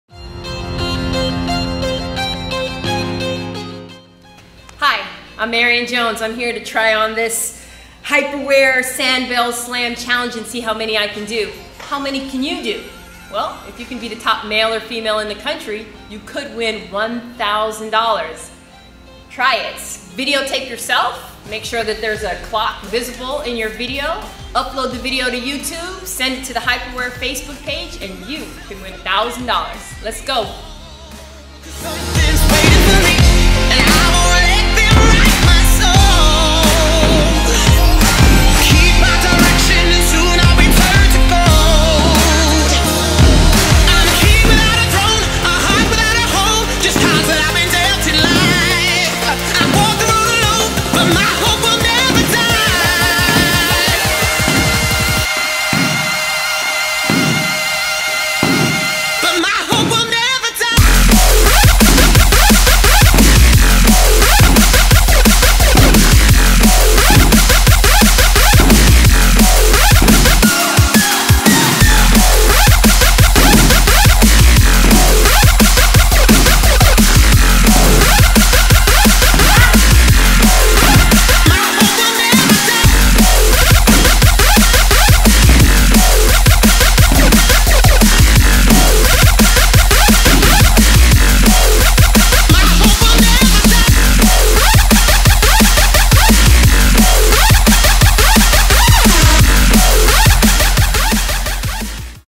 Marion Jones SandBell® Slam Challenge
Marion Jones takes on the Hyperwear SandBell Slam Challenge! Think you can beat her? Visit the SandBell Slam Challenge webpage for information on how to enter for the chance to win one of two $1000 prize packages.